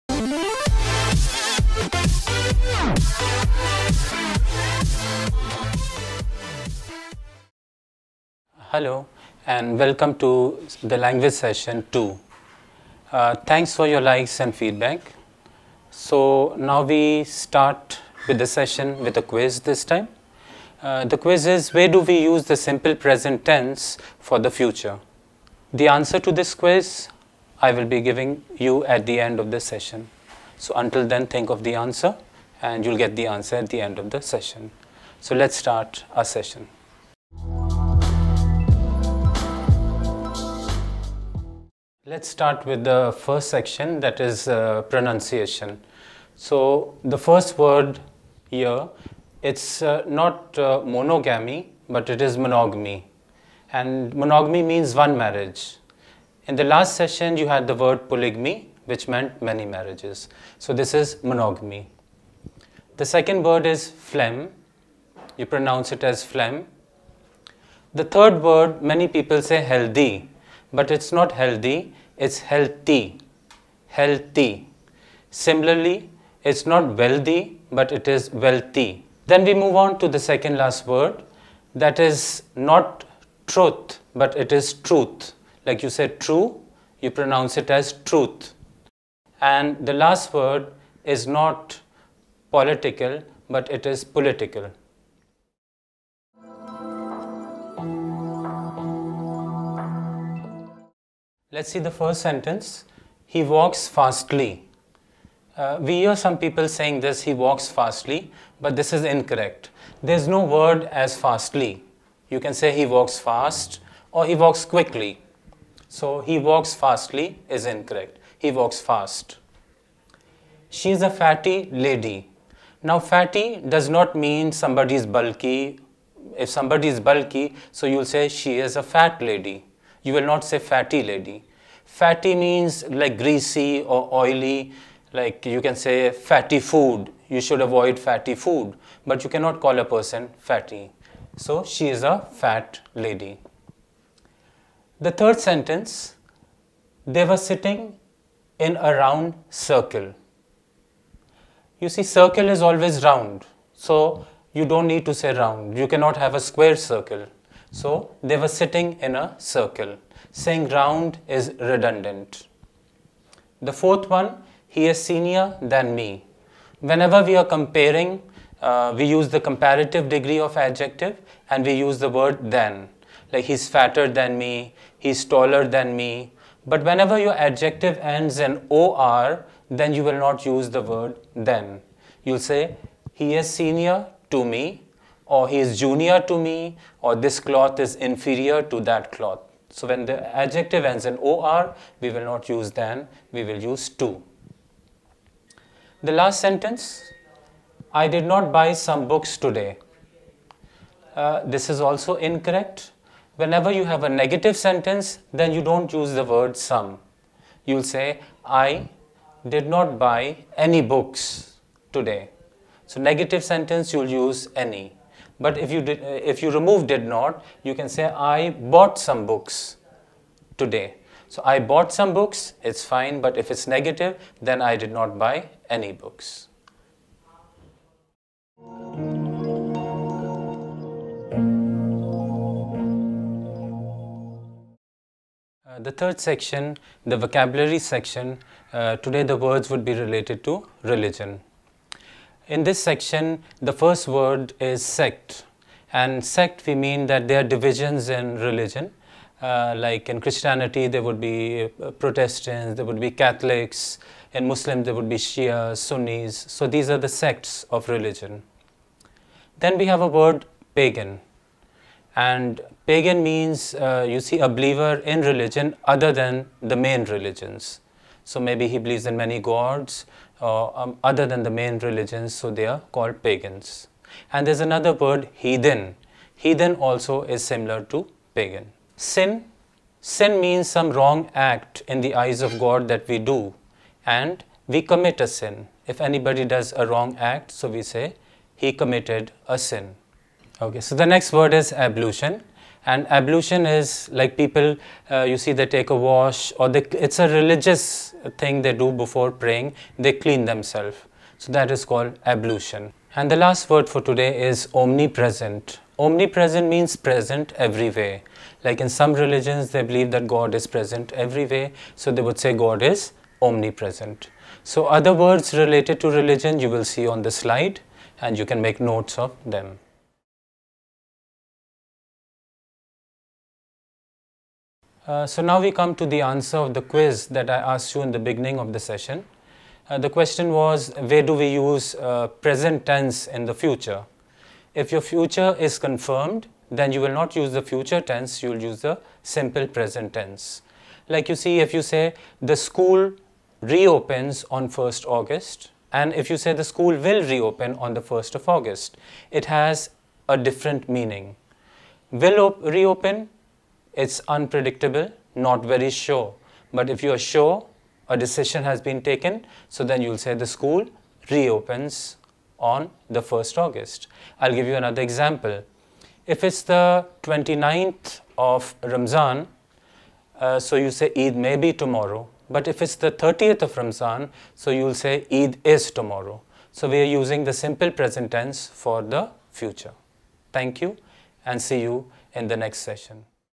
Lenny's English Language Session 2
An English Language video focusing on pronunciation(mispronounced words), error analysis(commonly made mistakes by people in English) vocabulary words and a quiz on grammar(using the simple present tense for the future)